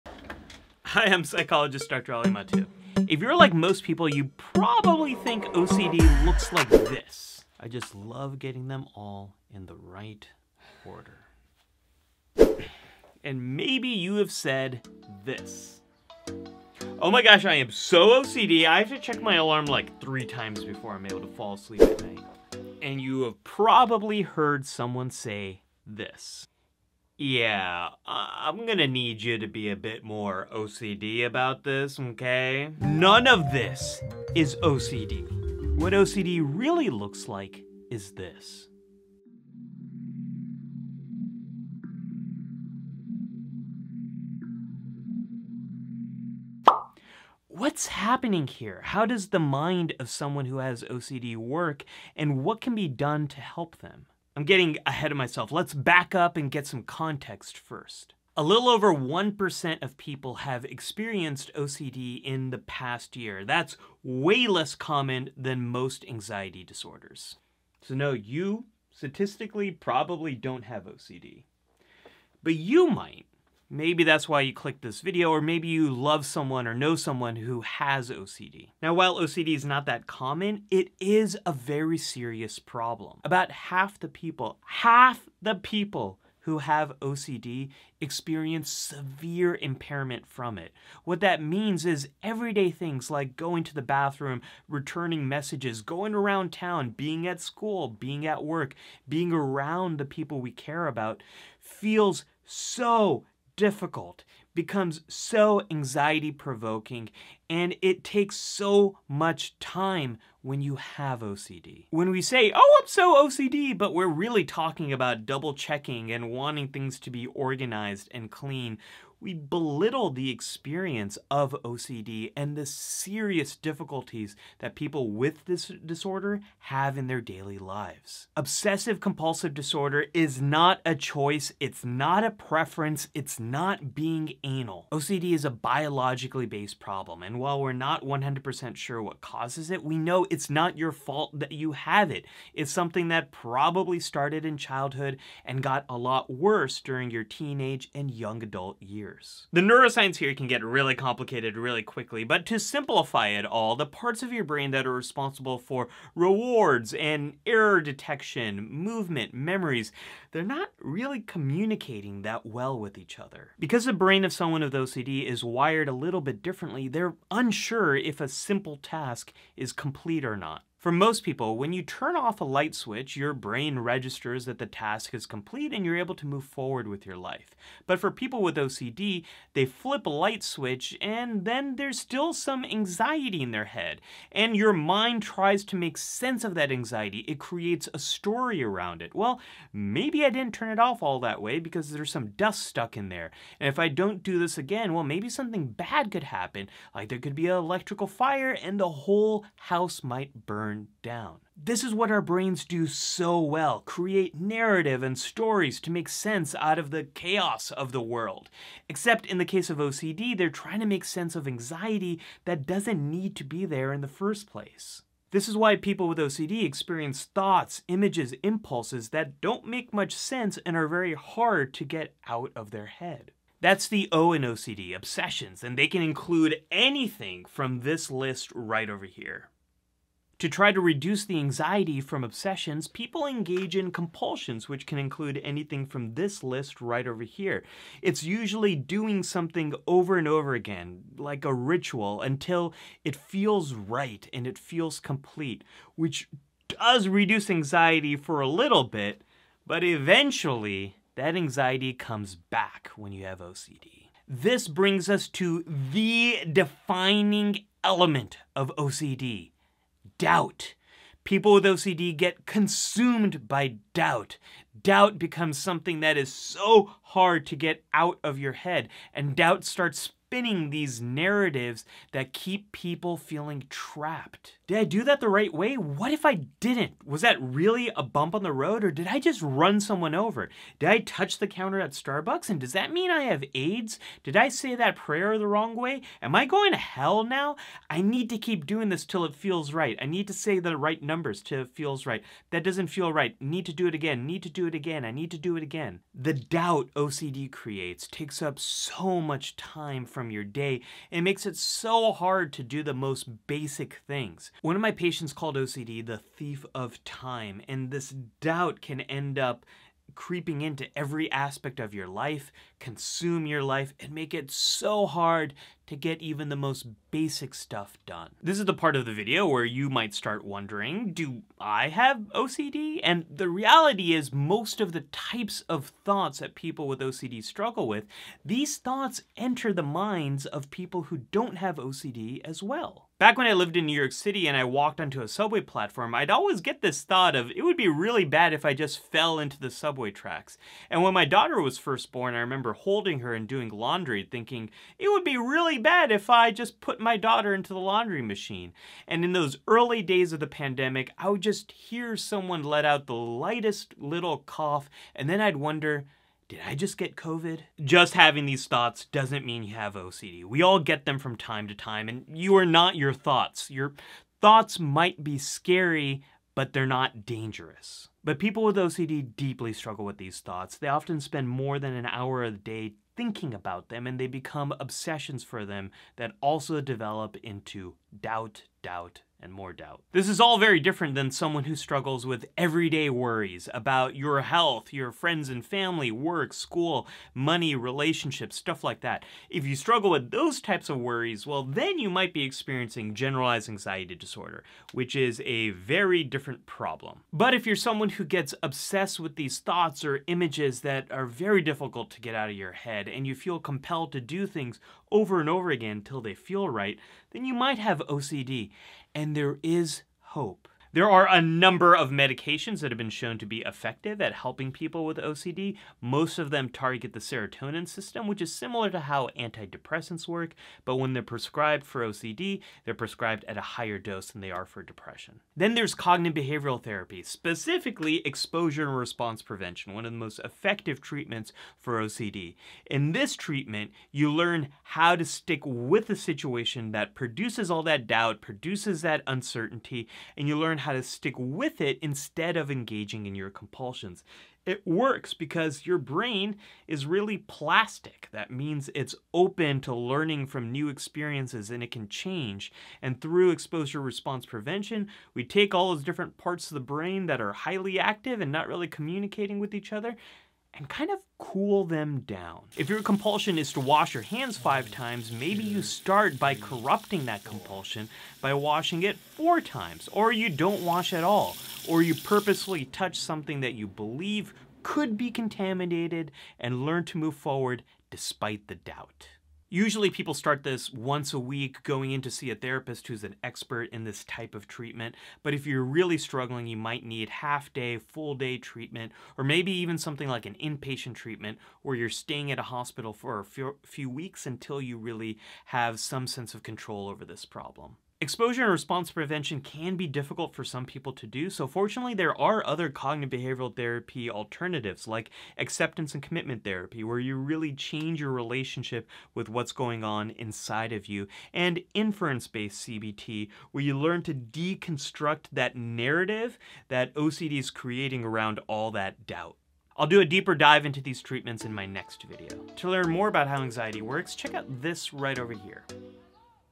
OCD explained for beginners - how I wish I was taught
How does the mind of someone with OCD (Obsessive-Compulsive Disorder) work and what can be done to treat this problem?

Contents:
0:00 - OCD stereotypes
0:54 - What is OCD?
2:30 - Neuropsychology of OCD
3:36 - OCD symptoms
5:20 - Do I have OCD?
6:50 - OCD treatment

If you or someone you know needs help immediately, you should take one of the following actions:
- call 9-8-8 in the United States or your country's emergency number
- call the Lifeline at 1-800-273 TALK (8255) in the United States or a global crisis hotlines
- go to your nearest hospital emergency room